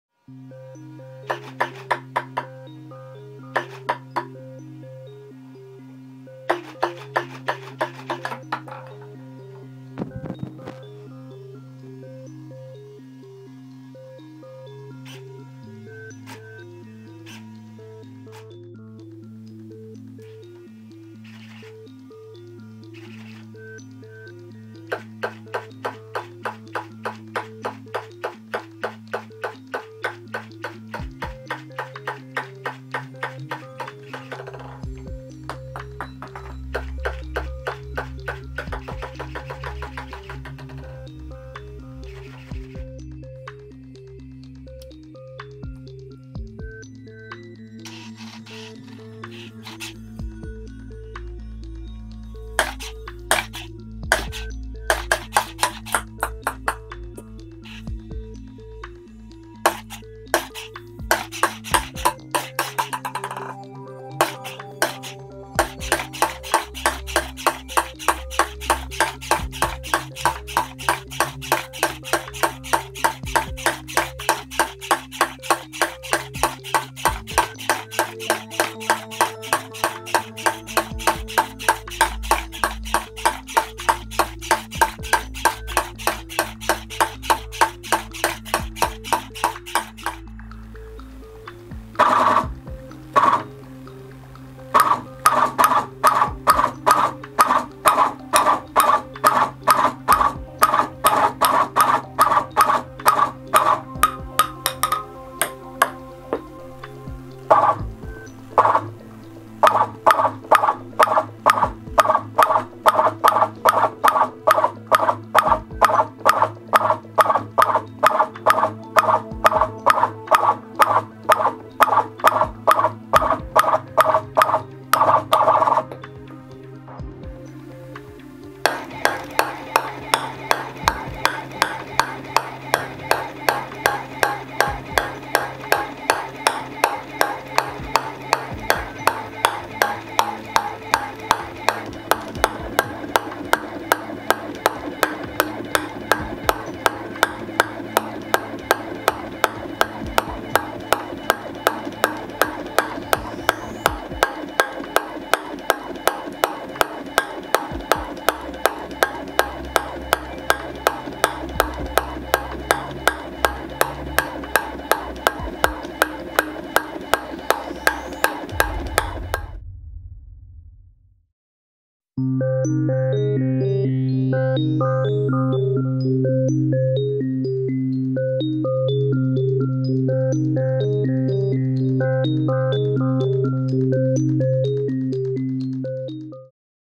The Evolution of an Arduino Project
Watch an amazing Arduino project evolve over the timespan of 4 years.

The builds in this video use Nema17 stepper motors, micro servos, Phototransistors, Arduinos, Raspberry Pis, and electret mics.